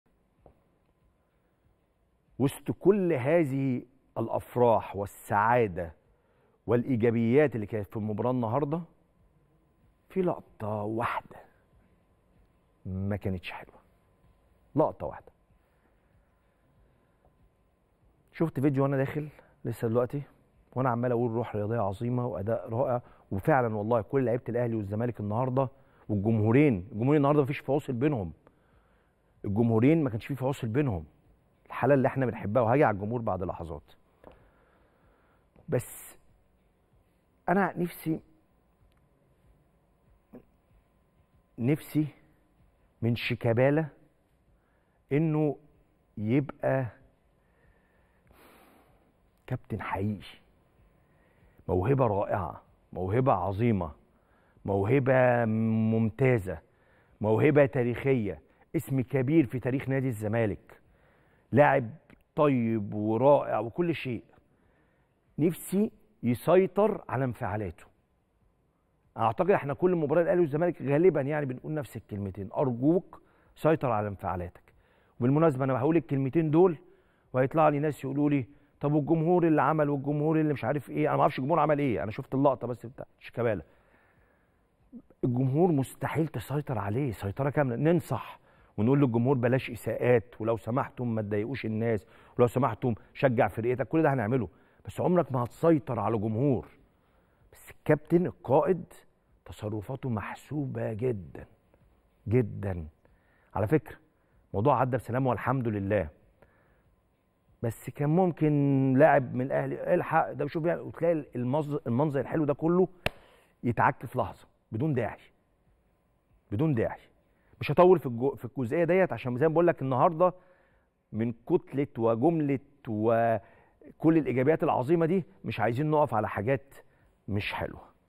"نفسي يبقي كابتن حقيقي.. ونفسي يسيطر على انفعالاته" 💥💥كريم رمزي يوجه رسالة خاصة لـ شيكابالا 👇🏻
"نفسي يبقي كابتن حقيقي.. ونفسي يسيطر على انفعالاته" 💥💥كريم رمزي يوجه رسالة خاصة لـ شيكابالا بعد المشادة مع جماهير النادي الأهلي 👇🏻